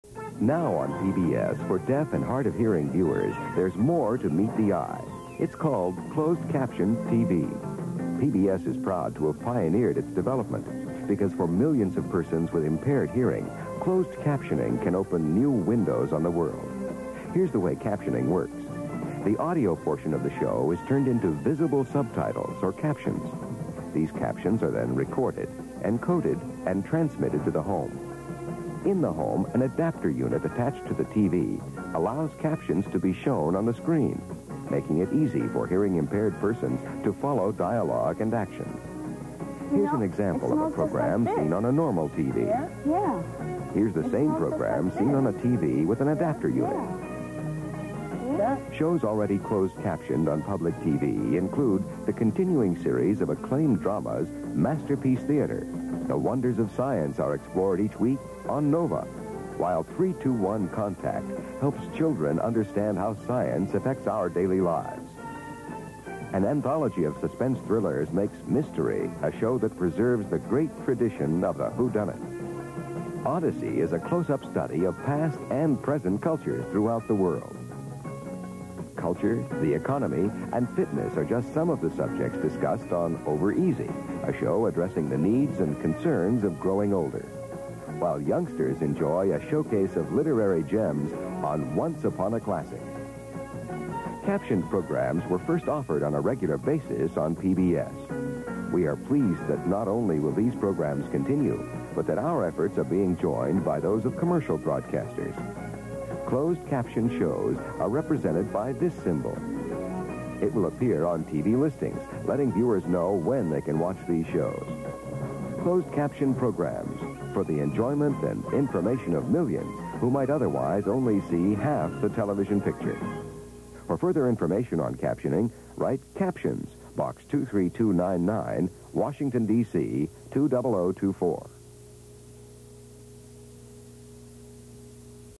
1981 PBS Closed Captions promo
Description of this then-new technology which came into use in 1980. You used to need a big external decoder to see them, it wasn't until the mid-90s when all TVs were required to decode them on their own.